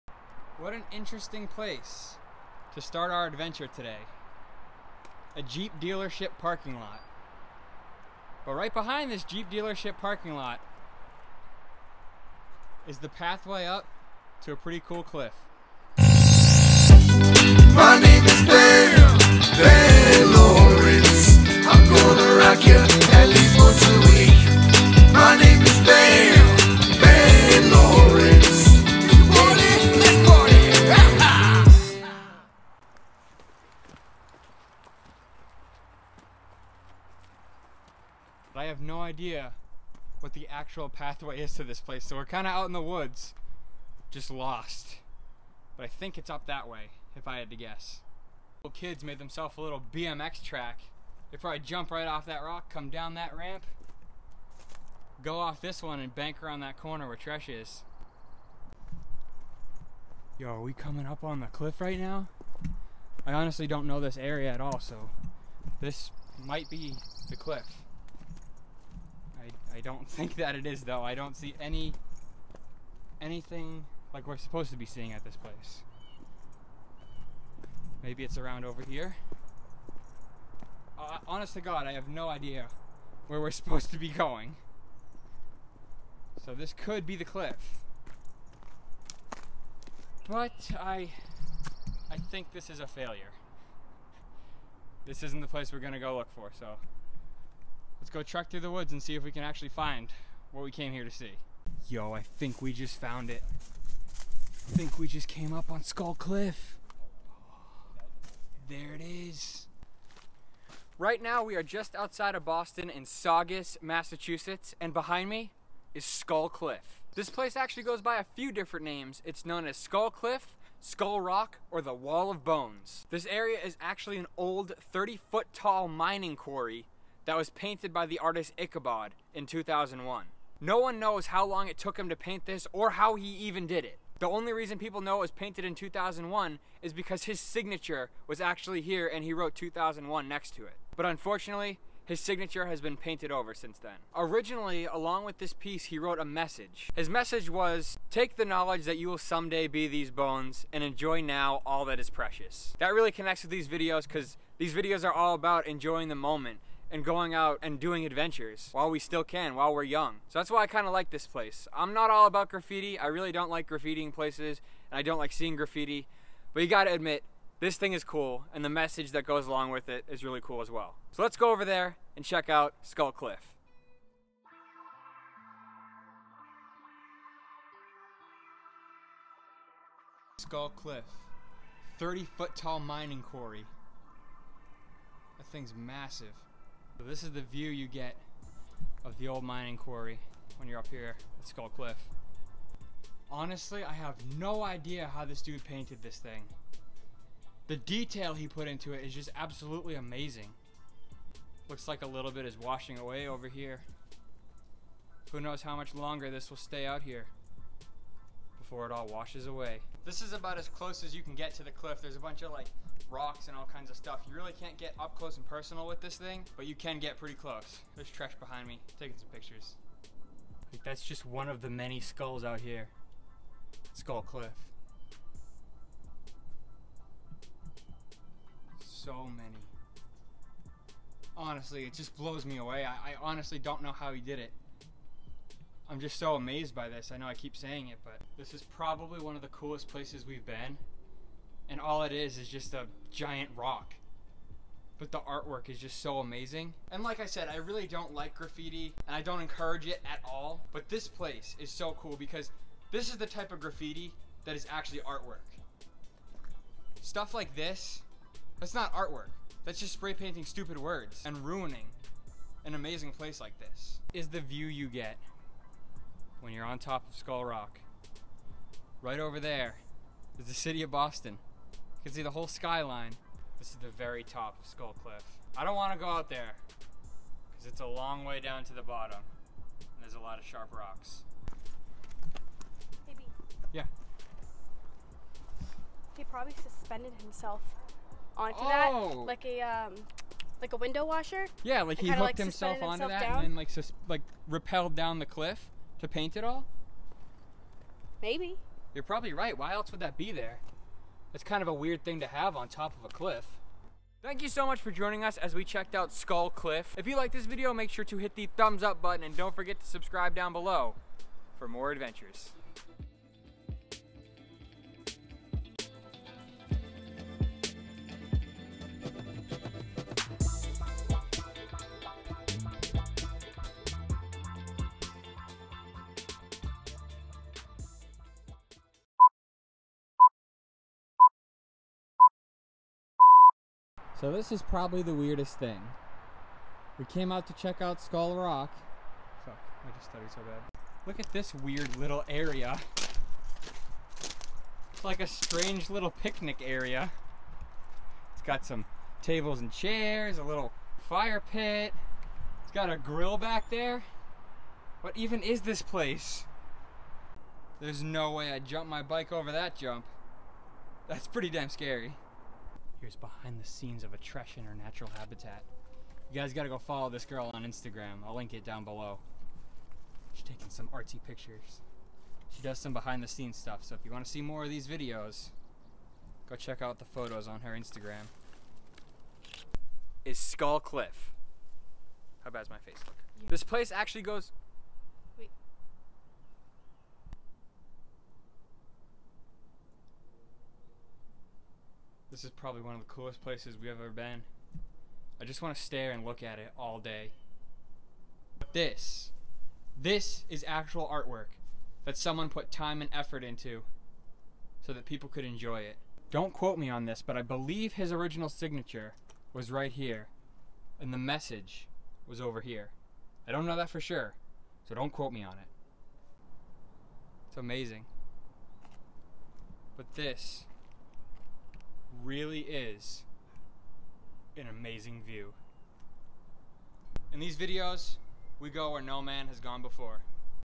Skull Cliff - Wall of Bones in Boston , Massachusetts
The Wall of Bones, a.k.a. Skull Cliff or Skull Rock, is located just off of Route 1 in Saugus Massachusetts which is just outside of Boston. This graffiti artwork was painted by the street artist known as Ichabod some time around the year 2001. Ichabod left behind this message with his painting:  “Take the knowledge that you will someday be these bones and enjoy now all that is precious.”

➤ Download the full theme song and other great songs on Rossco's SoundCloud